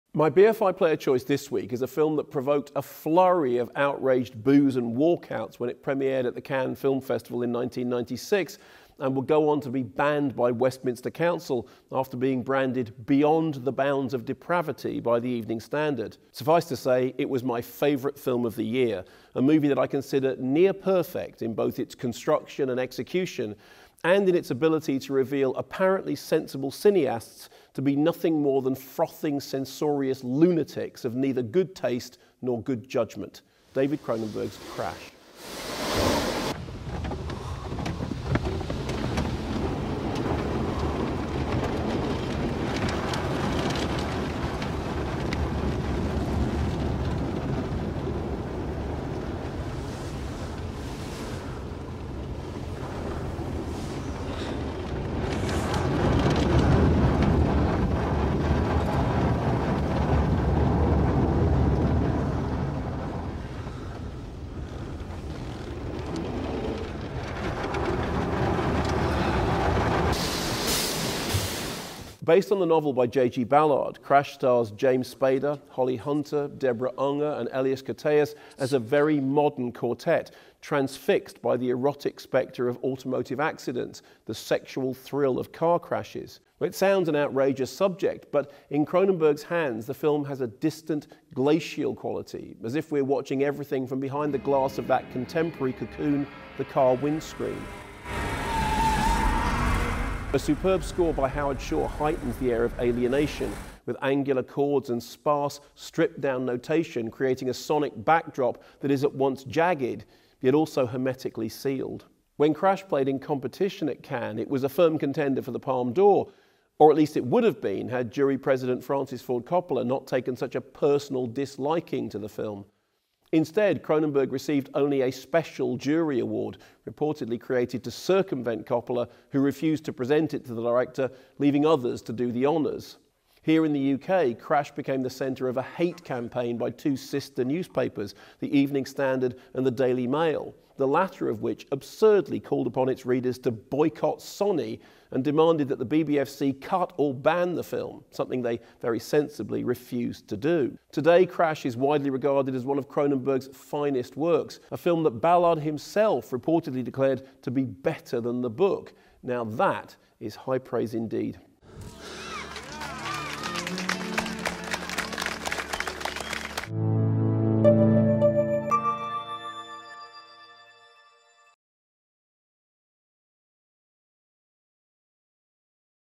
Mark Kermode reviews Crash (1996) | BFI Player
"A movie that I consider near-perfect in both its construction and execution."